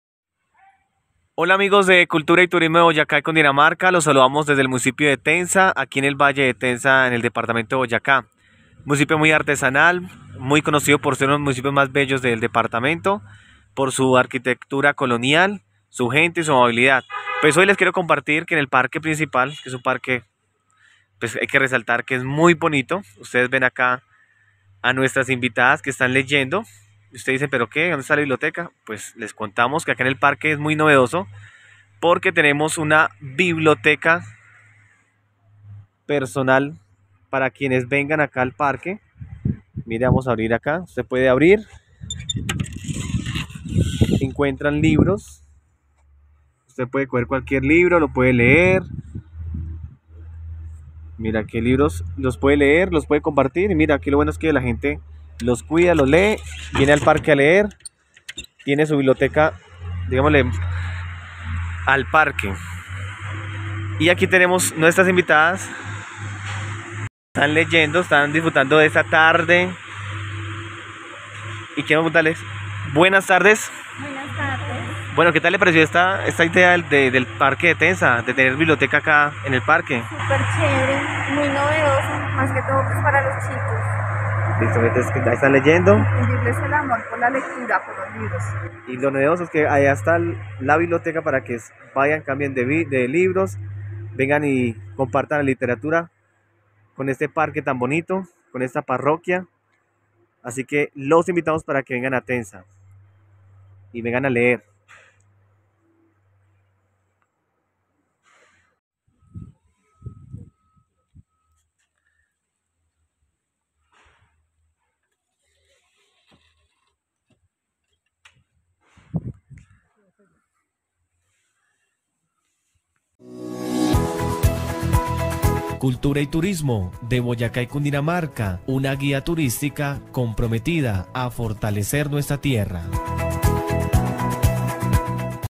Biblioteca En El Parque Principal De Tenza, Boyacá.
RecorridoCultour || El municipio de Tenza es conocido por su arquitectura colonial, por sus artesanías, por su gente amable y trabajadora; además por ser catalogado como uno de los municipios mas bonitos de Boyacá.

Hoy les queremos mostrar la estrategia para incentivar la lectura, a través de una biblioteca publica al aire libre, donde propios y visitante tienen la oportunidad de disfrutar de la literatura y de la tranquilidad de este hermoso municipio.

¡Muy bien por los creadores de esta estrategia!

Tenza es un municipio colombiano del departamento de Boyacá, situado en el centro-oriente de Colombia, en la región del Valle de Tenza. Está situado a unos 83 kilómetros de la ciudad de Tunja. Se ubica en la Provincia del Oriente.

El municipio limita por el norte con LaCapilla y Pachavita, al occidente con el departamento de Cundinamarca, al sur con Sutatenza, al oriente con Garagoa.

Dirección:
J. Alejandro Quintero
Gerente